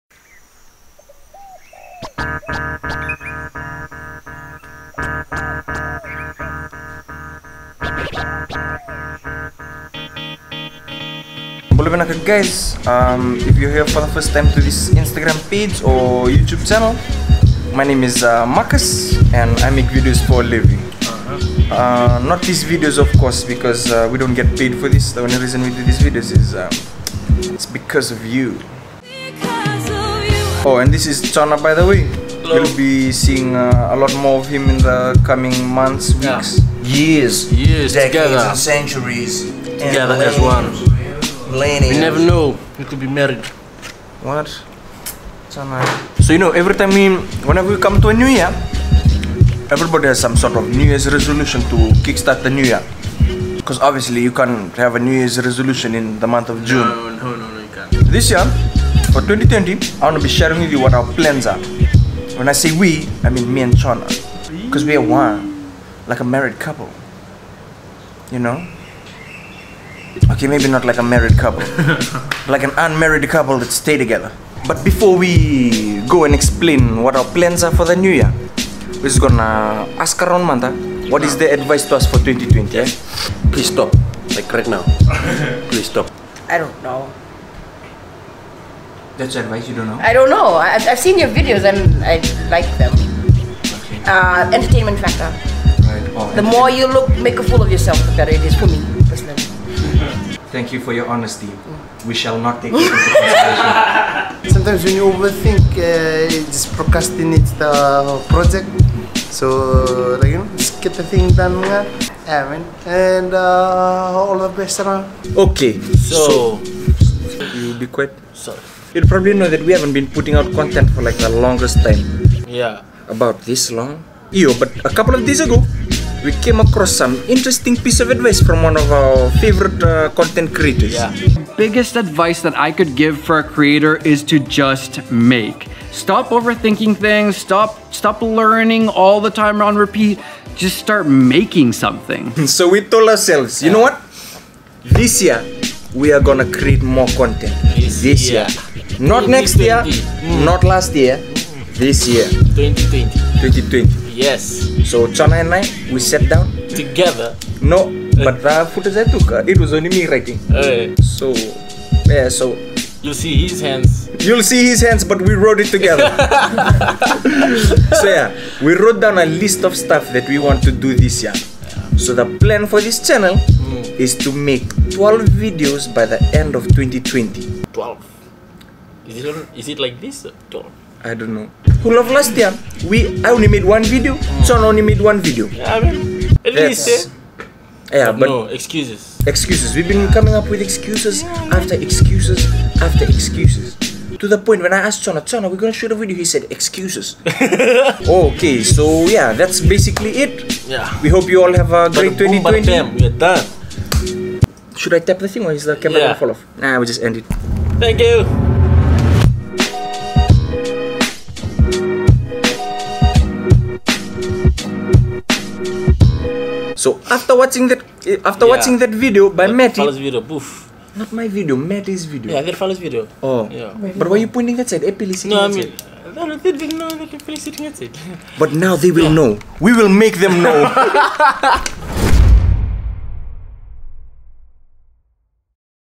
The 2020 Plan
Happy New Year guys. We made this video to hold ourselves accountable for our plans in 2020. We're glad to take you guys along on this journey. Hope you guys enjoy it.